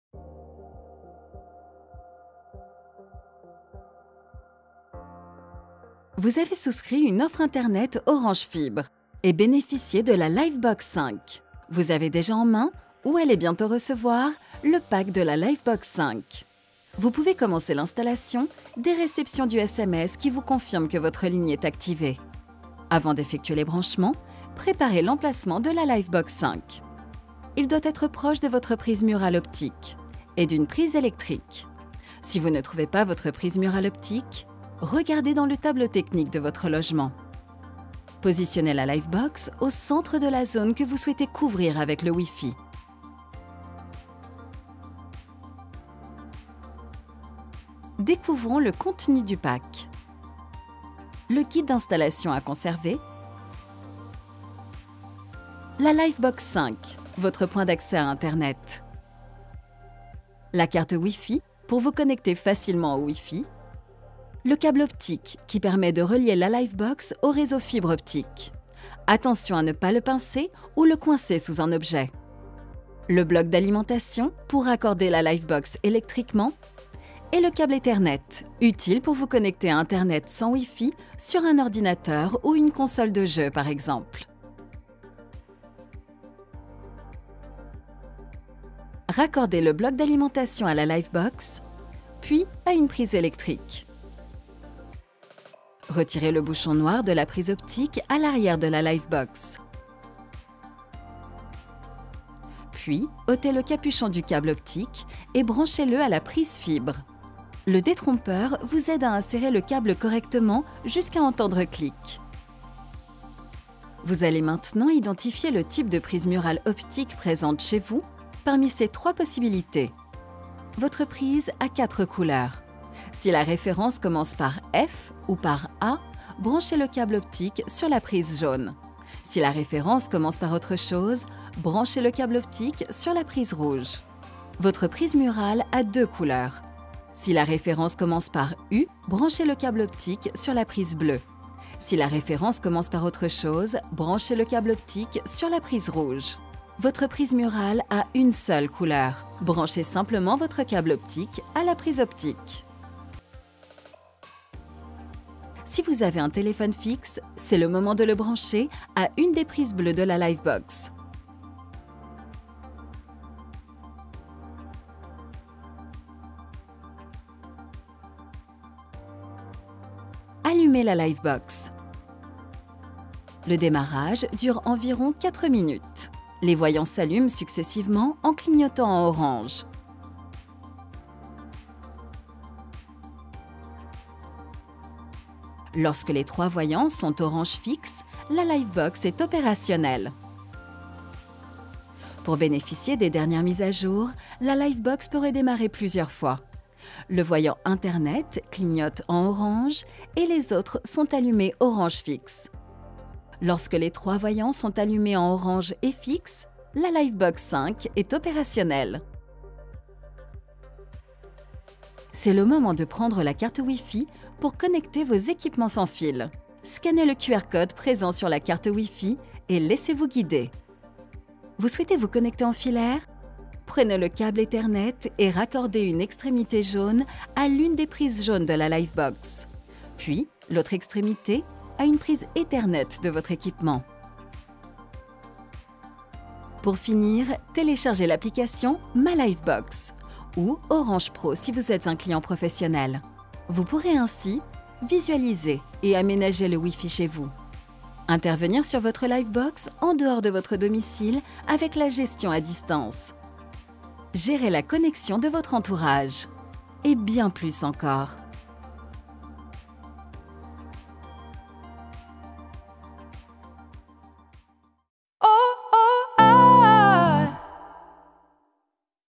Installer la Livebox 5 avec la Fibre d'Orange
Cette vidéo présente les différentes étapes d'installation de la Livebox 5 :
- réaliser les branchements,
- connecter votre smartphone, ordinateur, console de jeux, etc.
- télécharger l'application Ma Livebox.